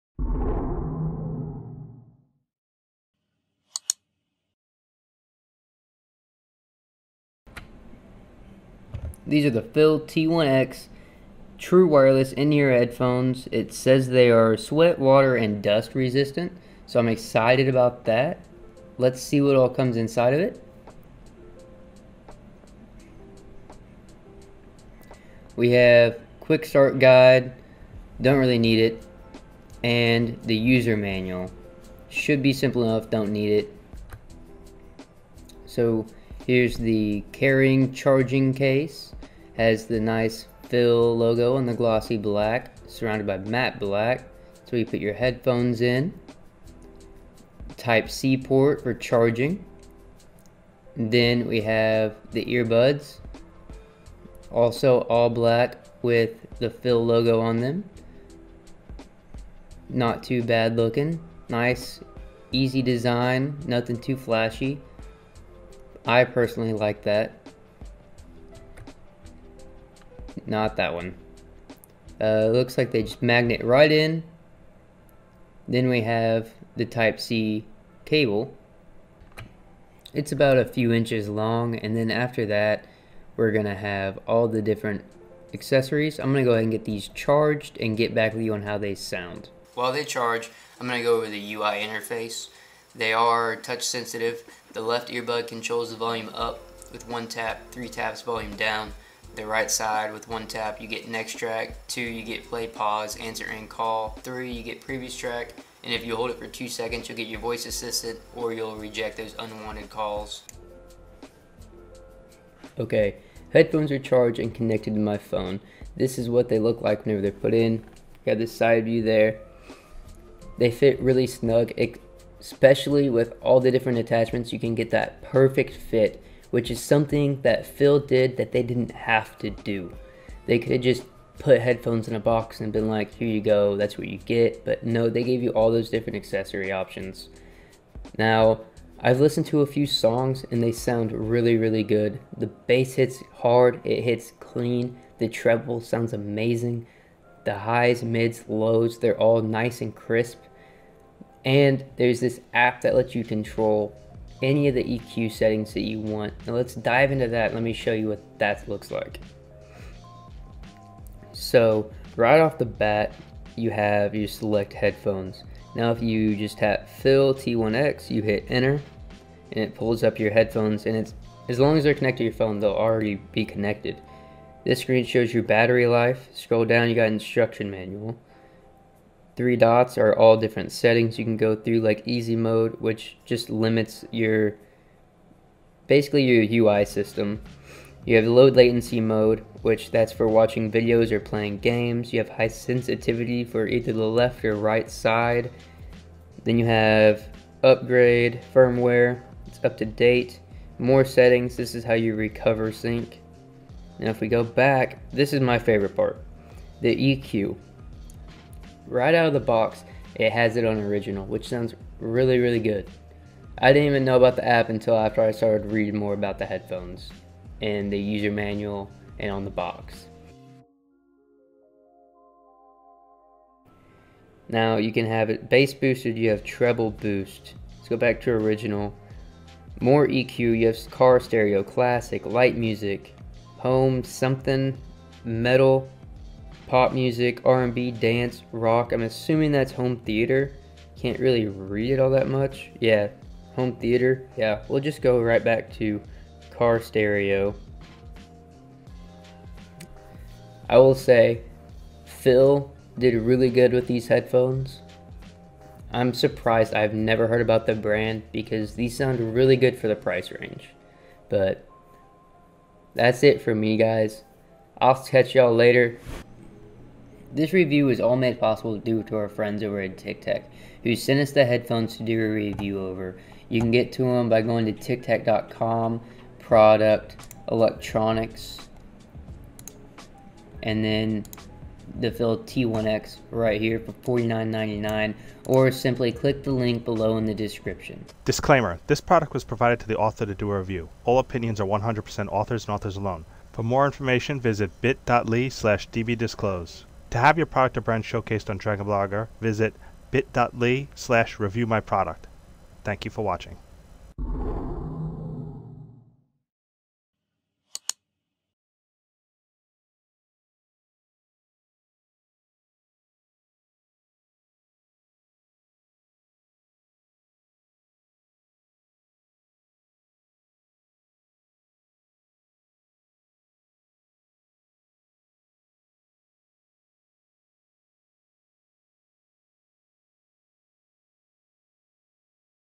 Fiil T1X True Wireless Bluetooth Earbuds Review
Briley reviews the Fiil T1X True Wireless Bluetooth Earbuds which were provided to us by the Tiktech store so that we could do a thorough and accurate review of them. Overall the Fiil T1X TWS Earbuds sound good and come with a larger variety of earbud tip choices than most brands provide.  The charging case works well, they fit in the ear well and sound very good.

Or become a member of our YouTube Channel (get a free random PC Steam game key too on membership)